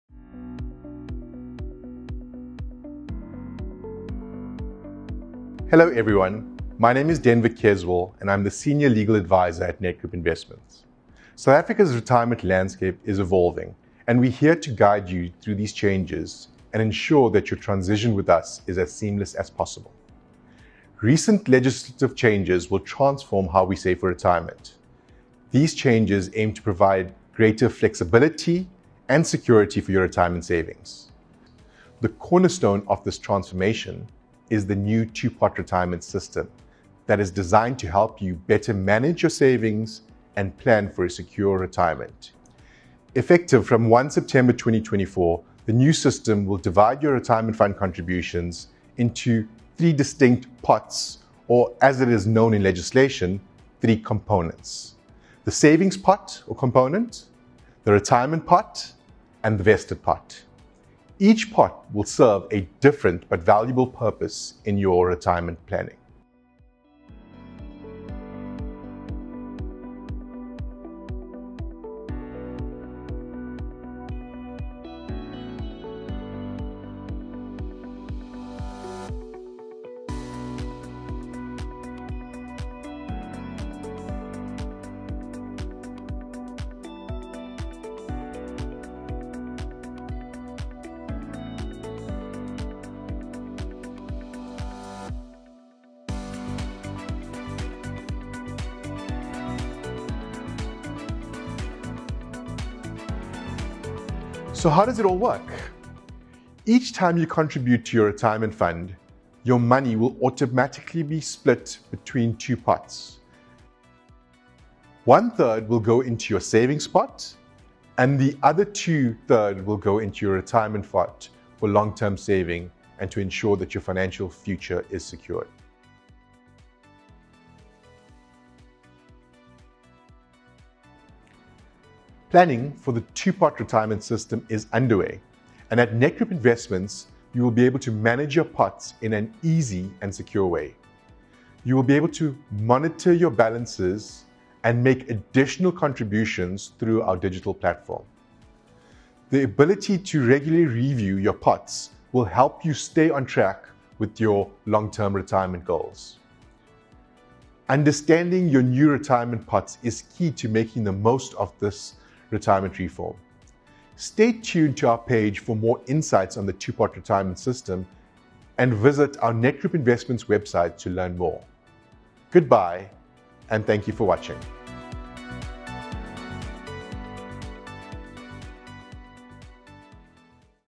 Meet your new retirement pots
The two-pot retirement system will come into effect on 1 September 2024. Meet your new retirement pots and learn more about what this means for your retirement planning with our Senior Legal Advisor, Denver Keswell.